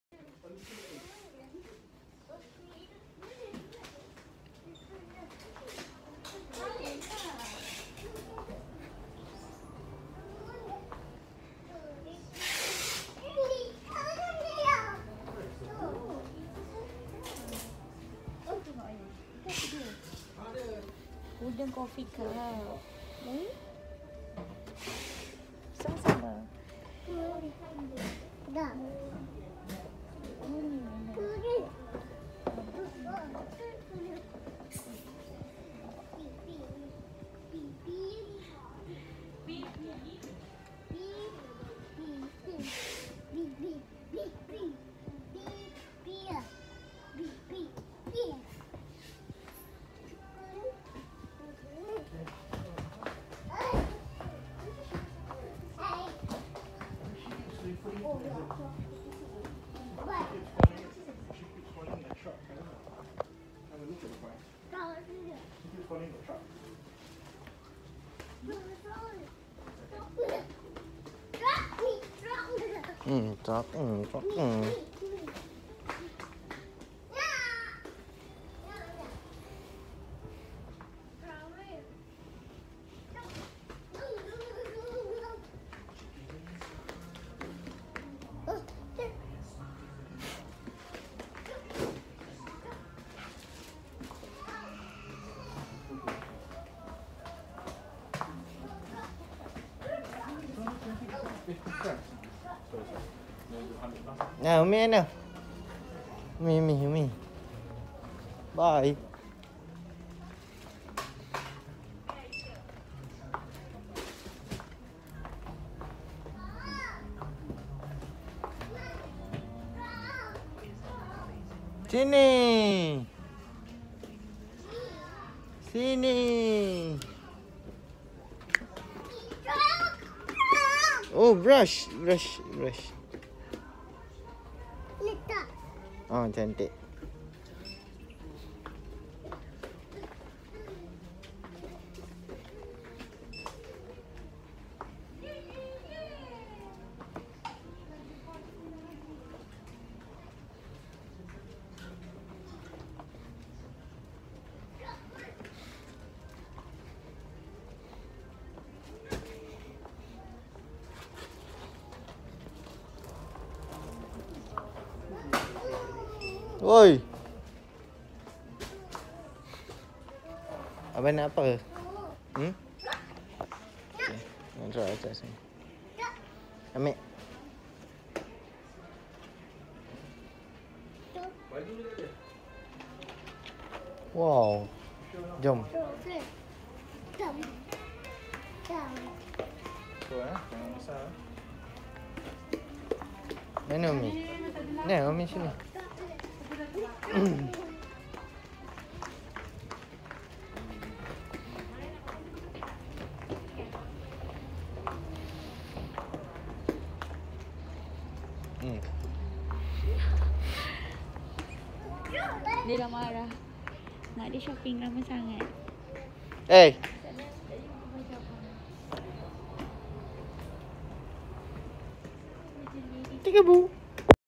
Mother care subang parade
Jalan jalan mother care
Umie cari baju abang dengan adik